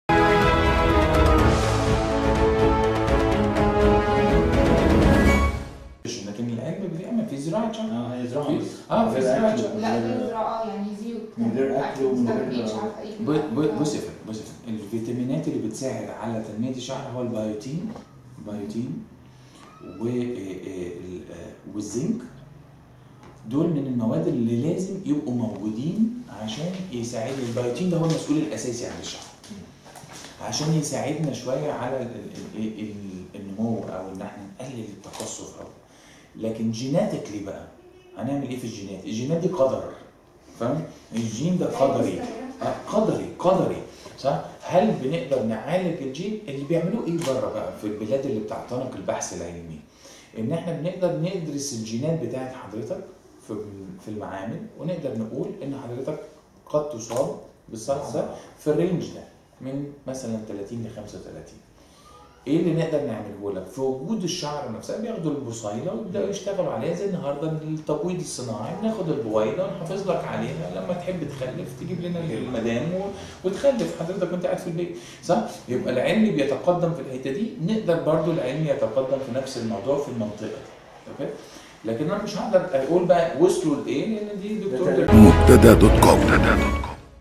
هانى أبوالنجا: البوتين والزنك لعلاج مشاكل الشعر
يتعرض الشعر للعديد من المشاكل اليومية، من التقصف للتساقط وحتى الصلع. سألنا الدكتور هانى أبو النجا عن العوامل المسؤولة عن ذلك، فكانت هذه الإجابة.

قال إخصائى التغذية العلاجية لـ"مبتدا": "الفيتامينات المساعدة على تنمية الشعر هى البيوتين والزنك، والبيوتين هو المسؤول الأساسى عن الشعر، ويساعد فى نموه ومنع تقصفه".

وأضاف مدير قسم التغذية العلاجية فى عيادات "دايت هوم"، الحاصل على الدكتوراه فى علوم التغذية: "وهناك جينات مسؤولة عن الصلع، ومؤخرًا أصبح بالإمكاىن دراسة الجين وتحديد فترة الصلع، وبالتالى أخذ البصيلة ومعالجتها، أو الحفاظ عليها، لزراعة شعر منها، مثل الحفاظ على البويضة".